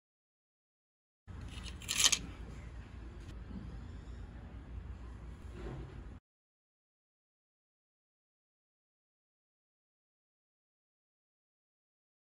Abalone shell in Luxitune fine tuners
Have you ever had Abalone? Well our creative director HyeJun is visiting in Korea and she had some for dinner and is keeping the shells because of their stunning beauty and because how cool is it to see the materials we use to make Luxitune tuners in its natural form?
Here we include the actuall Abalone shells as well as the two tuners we have that utilize the Abalone shell material:

- Dark Grey Mother of Pearl fine tuner
- Antique inspired hand engraved Octagon shape Abalone shell inlay tuner